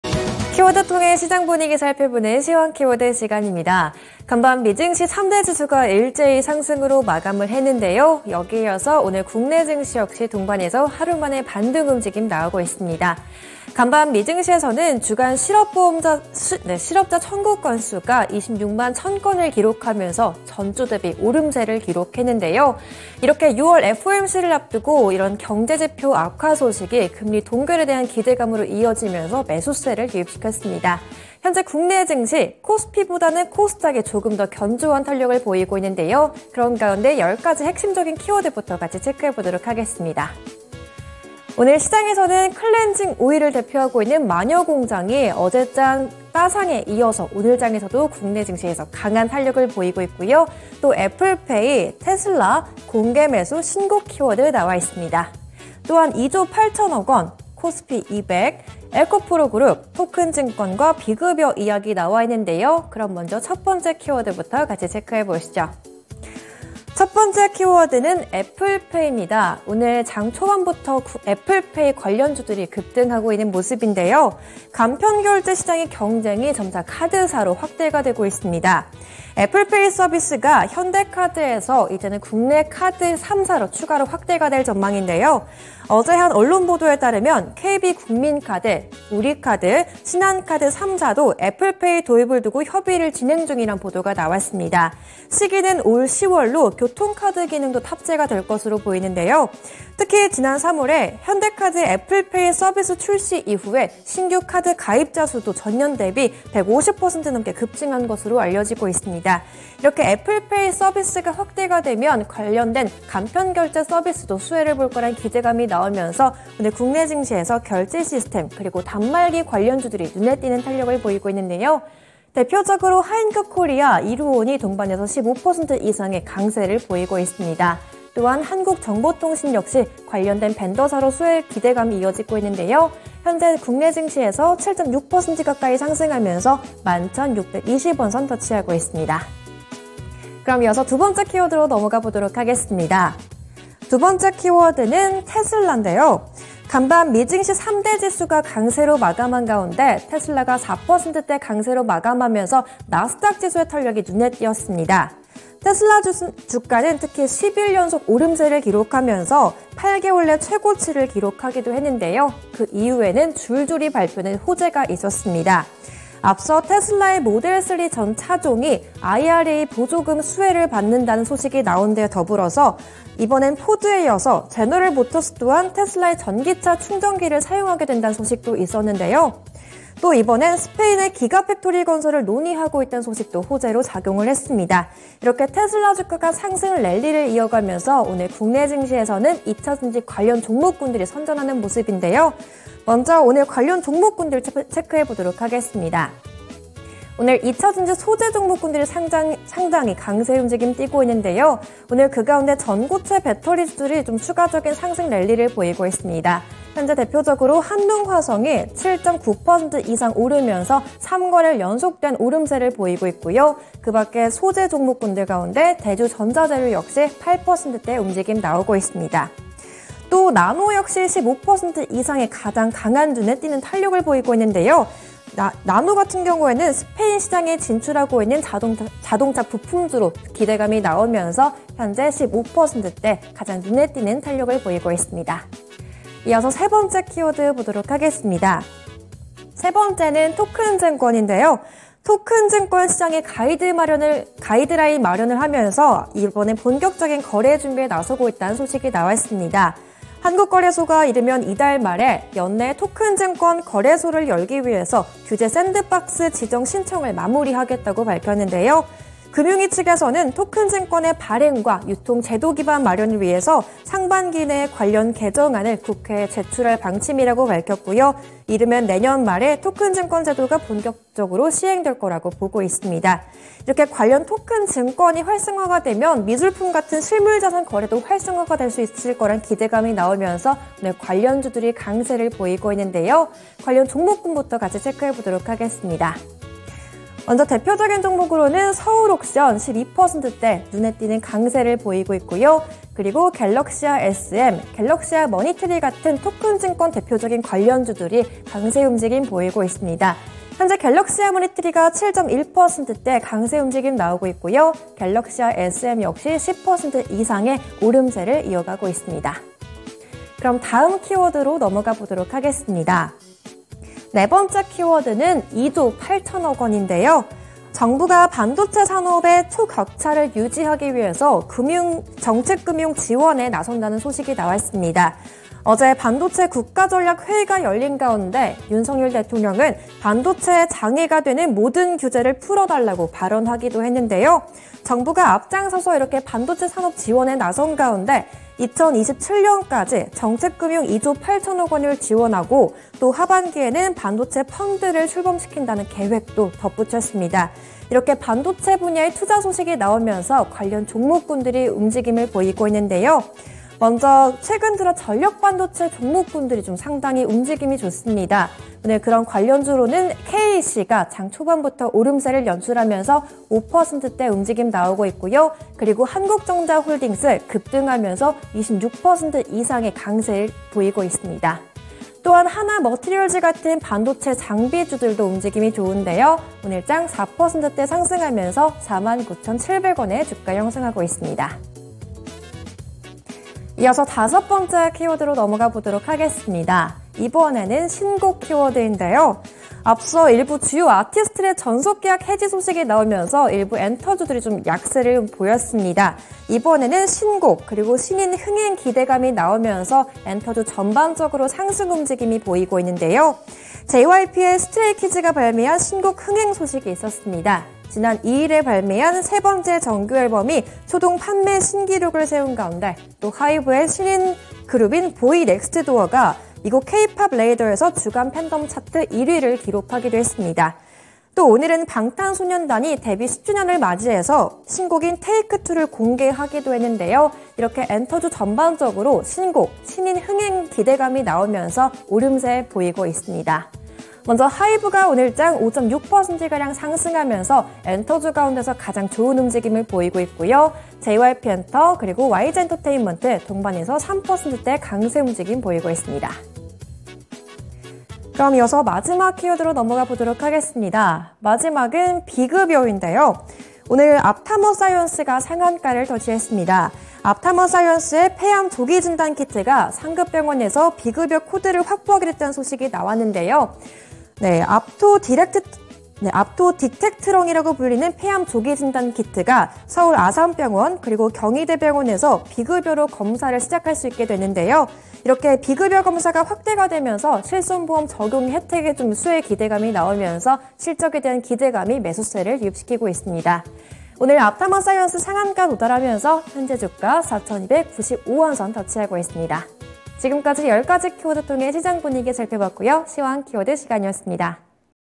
애플페이/테슬라/토큰증권/2.8조/신곡/비급여/공개매수/코스피200/에코프로그룹/클렌징 오일 [시황키워드_머니플로우11]
▶시황 키워드
▶캐스터: 조수연
▷ 키워드 : 애플페이/테슬라/토큰증권/2.8조/신곡/비급여/공개매수/코스피200/에코프로그룹/클렌징 오일

1) 애플페이 관련주, 카드사 ‘3곳’ 추가 합류 소식에 급등 (하인크코리아/이루온/한국정보통신)

2) 테슬라 주가 상승랠리에 2차전지주 강세 (한농화성/대주전자재료/나노)

3) 거래소, 연내 토큰증권 거래소 출범 소식에 관련주 상승세 (갤럭시아에스엠/갤럭시아머니트리/서울옥션)

4) “반도체 초격차 유지”...2.8조 정책금융 지원 소식 속 관련주 강세 (한국전자홀딩스/KEC/하나머티리얼즈)

5) 신곡 흥행 기대감 속 엔터주 강세 (하이브/JYP Ent./와이지엔터테인먼트)

6) 압타머사이언스, 상급병원서 폐암조기진단키트 비급여 코드 확보에 상한가